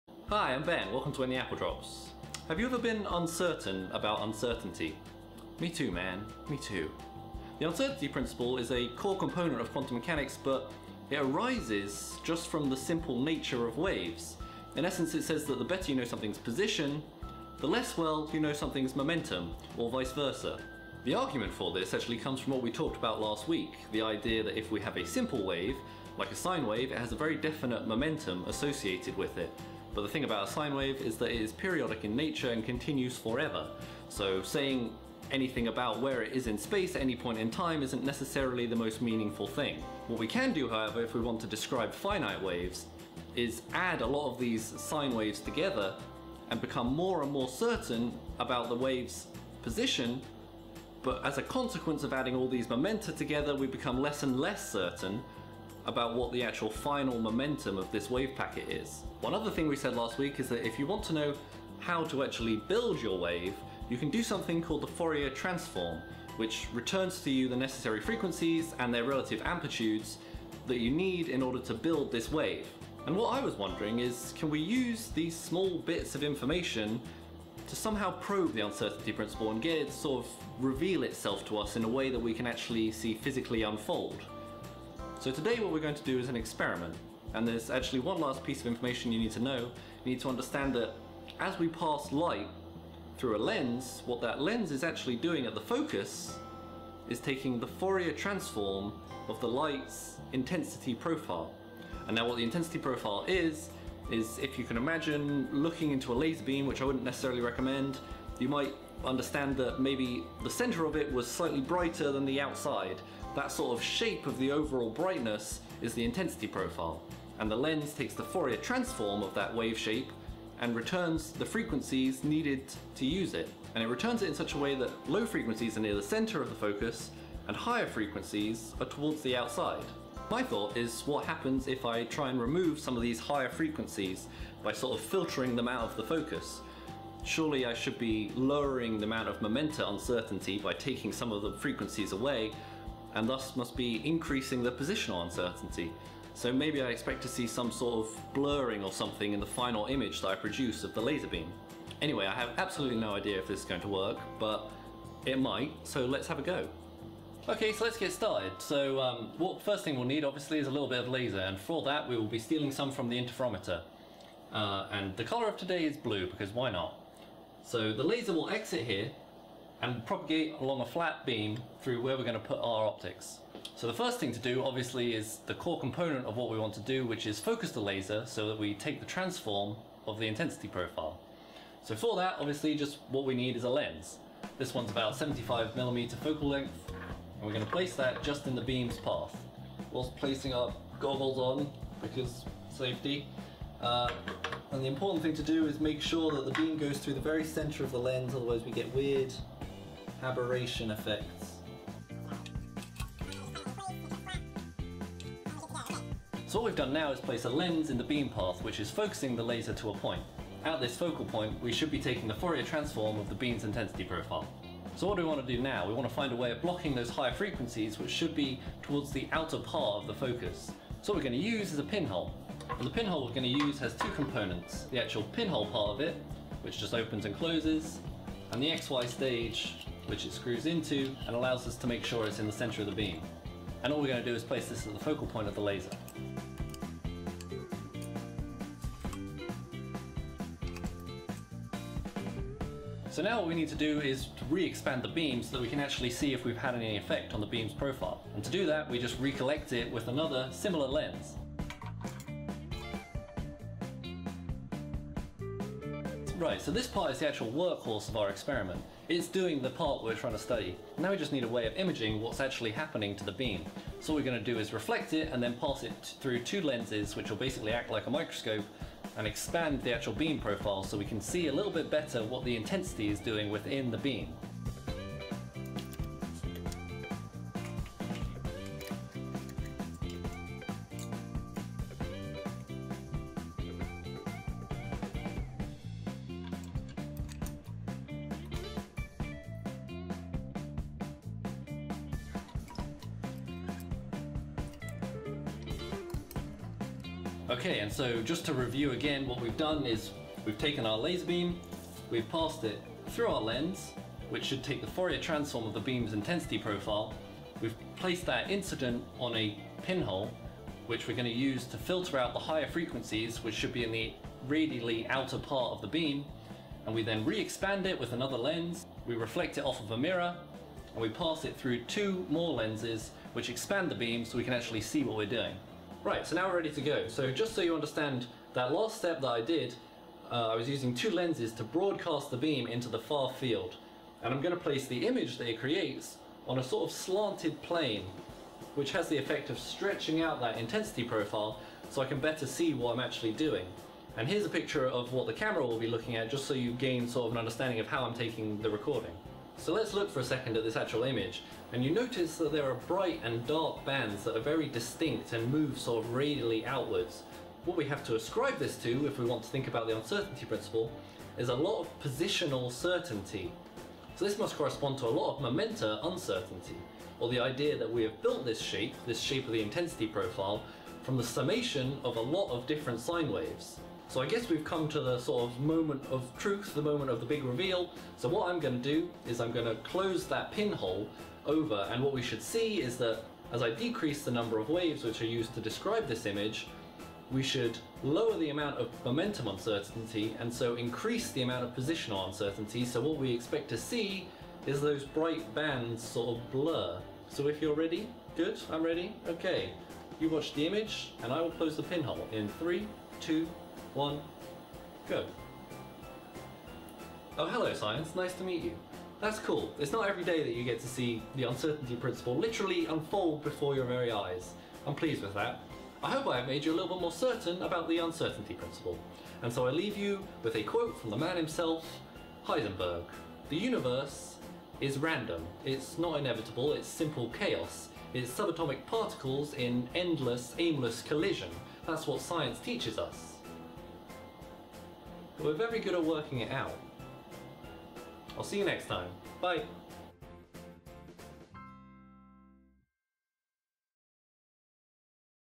Showing the Uncertainty Principle
How does the Uncertainty Principle Work?

This week, we take a look at Heisenberg's Uncertainty Principle. And attempt with reasonably but not spectacular success to demonstrate it in action using spatial filtering and laser beams.

This is a re-upload from yesterday as I wasn't happy with how the audio encoded (I did it wrong, cause I'm a wally).  Big thanks to Star Badger for pointing it out and OwenWithAHammer for testing it over different devices.  You guys are awesome.